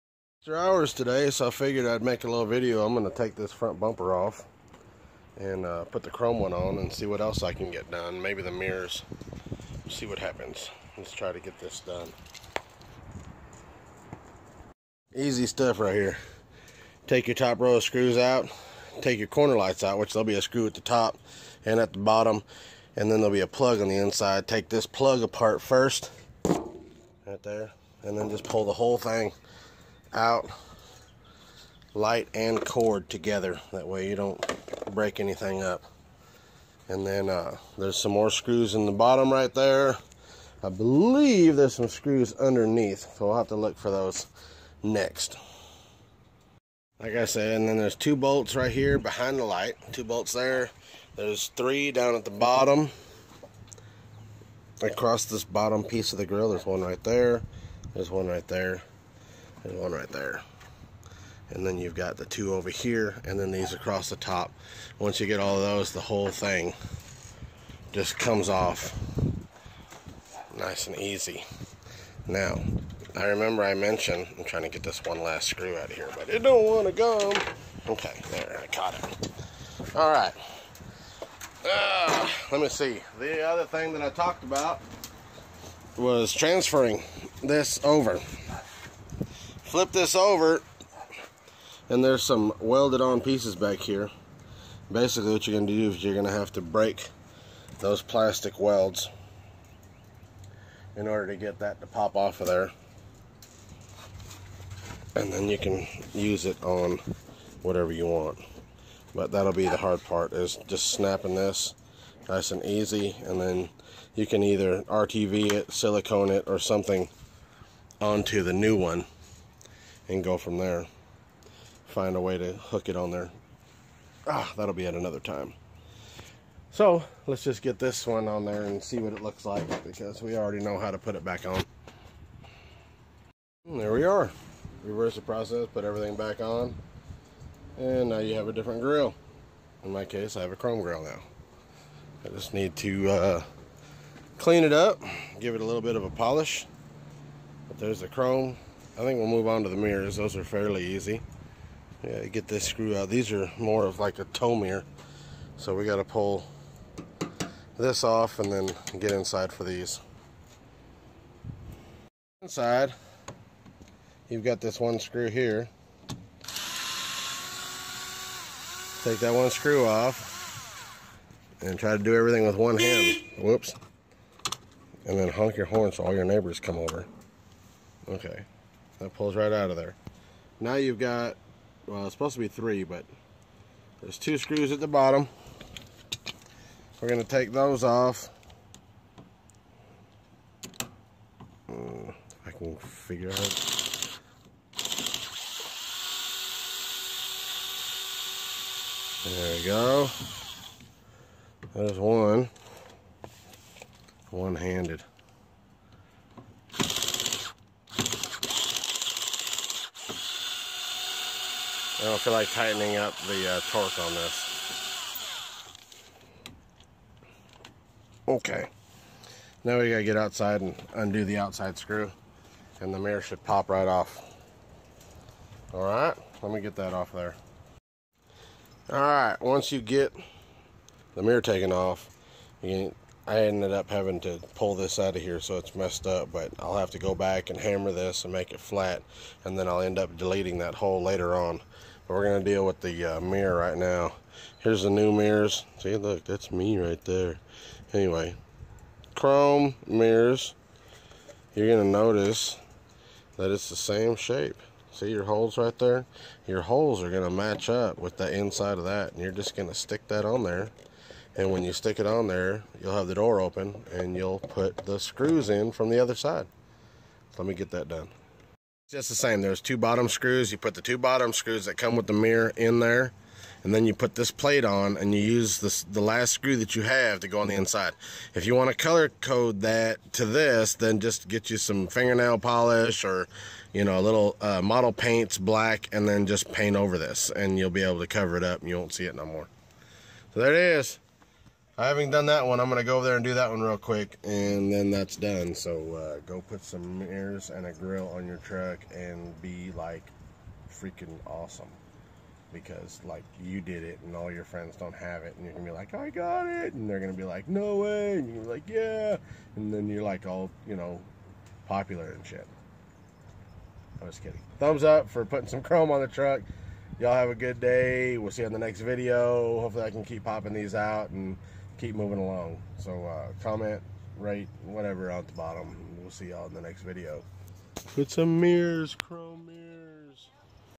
Installing grill and side mirrors
I go my chime grill and side mirrors installed.